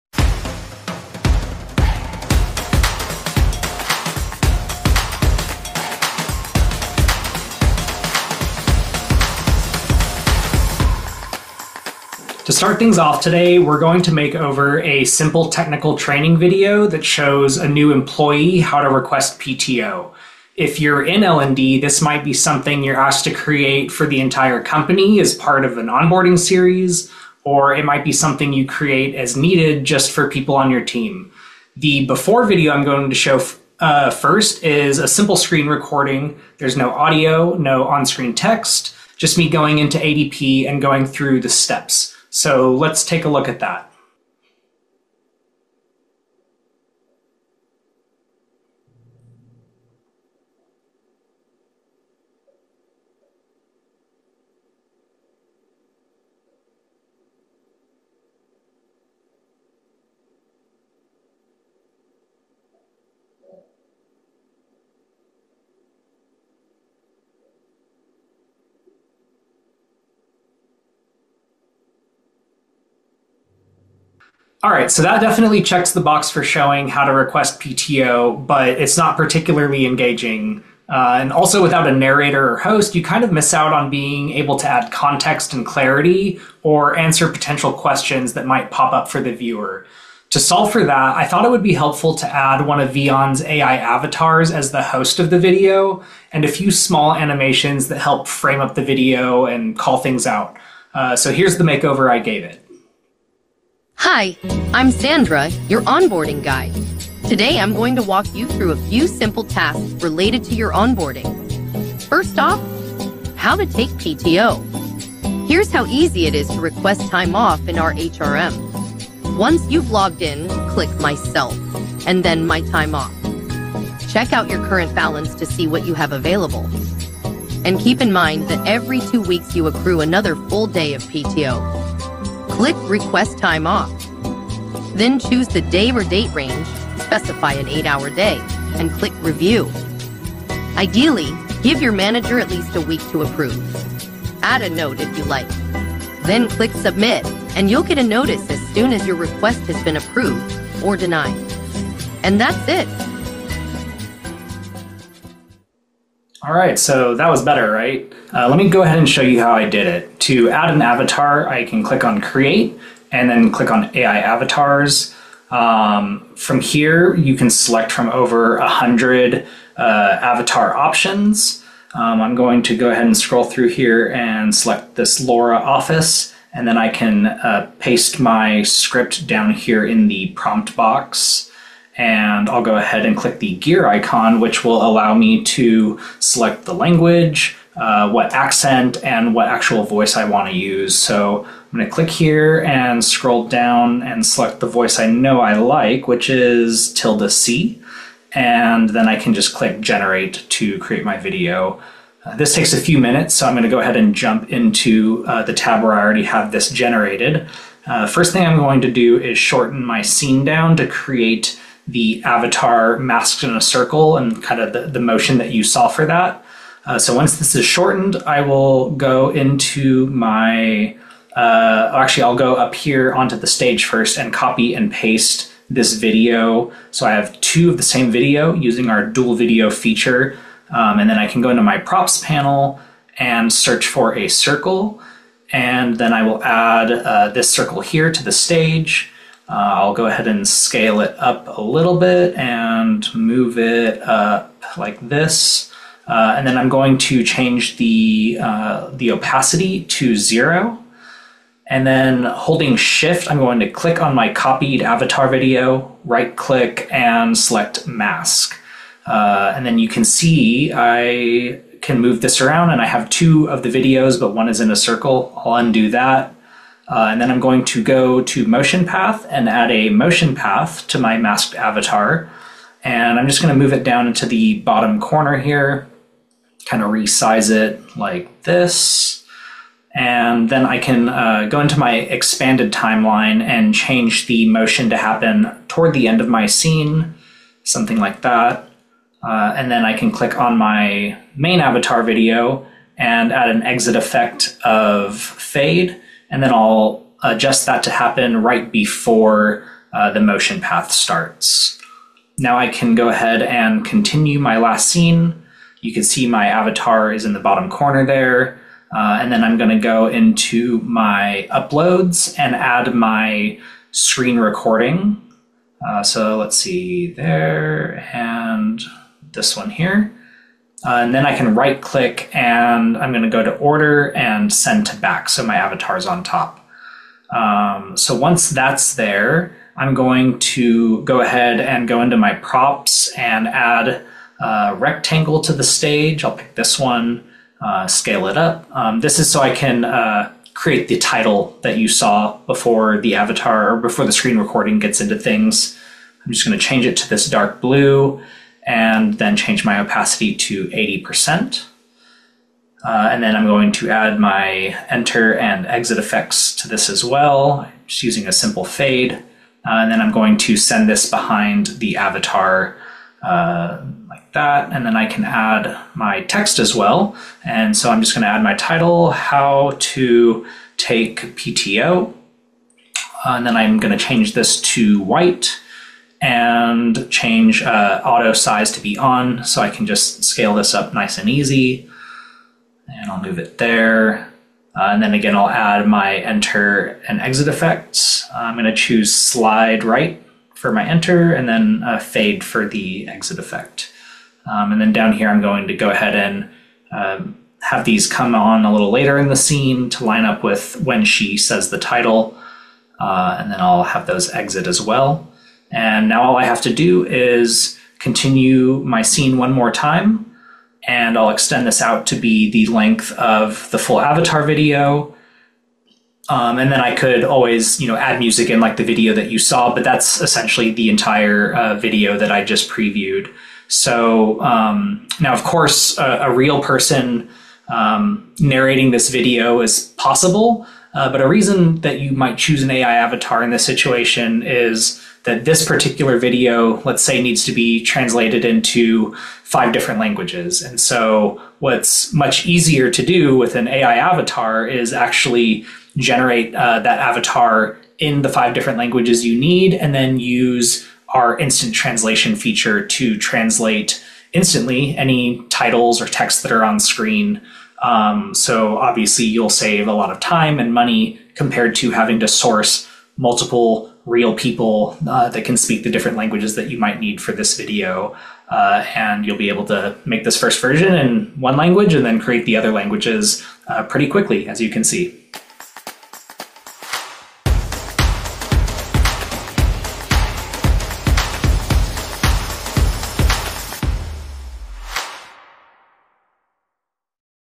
Add an Al Avatar Presenter to a Demo Video | Vyond Bootcamp Video Tutorials
See how to create and add an AI avatar to a simple screen recording in Vyond. Adding a presenter to any video has never been easier.

Find out for yourself how easy creating and using AI avatars is in Vyond.